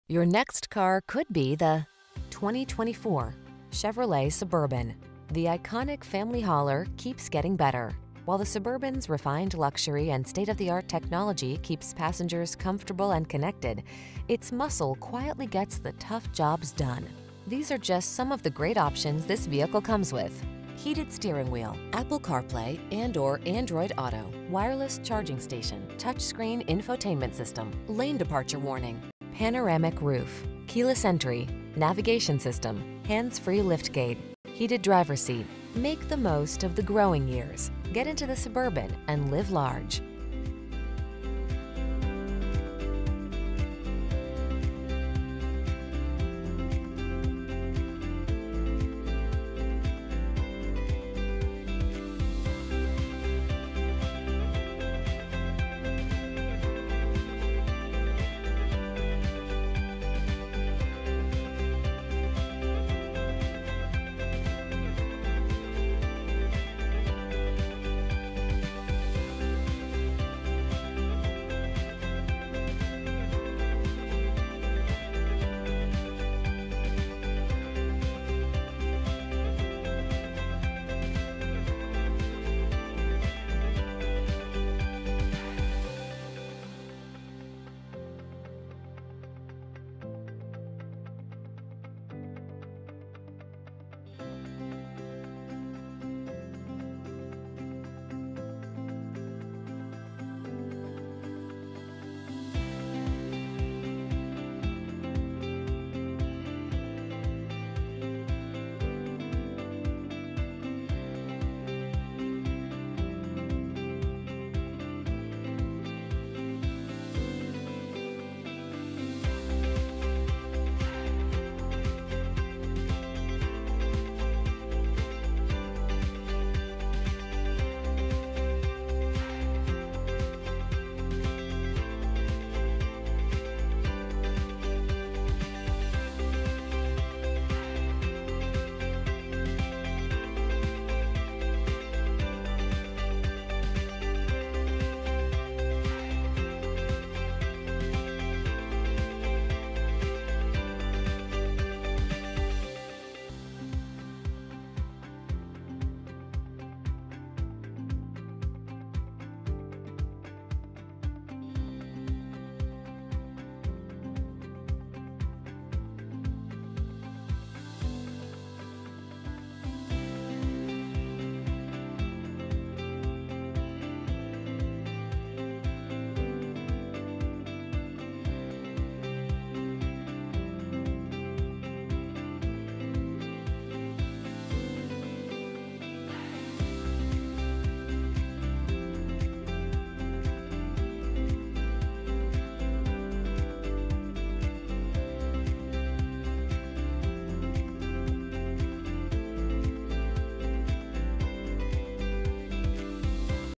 2024 Chevrolet Suburban Z71 Lake Bluff, Lake Forest, Libertyville, Waukegan, Gurnee IL
New 2024 Chevrolet Suburban Z71 available at Chevy Exchange located in 1 Sherwood Terrace, Lake Bluff, IL, 60044. Servicing the Lake Bluff, Lake Forest, Libertyville, Waukegan, Gurnee area.

Your next car could be the  2024  Chevrolet Suburban. The iconic family hauler keeps getting better. While the Suburban's refined luxury and state-of-the-art technology keeps passengers comfortable and connected,  its muscle quietly gets the tough jobs done. These are just some of the great options this vehicle comes with: Heated Steering Wheel
, Apple Carplay®/Android Auto®, Wireless Charging Station, Touchscreen Infotainment System, Lane Departure Warning, Panoramic Roof, Keyless Entry, Navigation System, HANDS-FREE LIFTGATE, Heated Driver Seat. Make the most of the growing years. Get into the Suburban and live large.